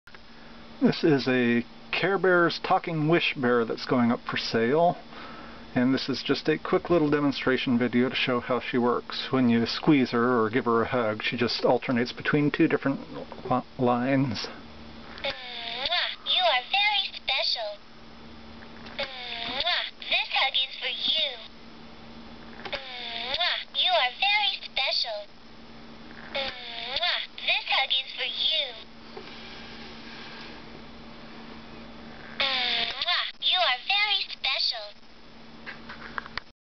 Sale Item Demo - Hugging Kissing Talking Care Bears Wish Bear
Very quick demo for a Care Bears talking, hugging, and kissing Wish Bear that is going up for sale.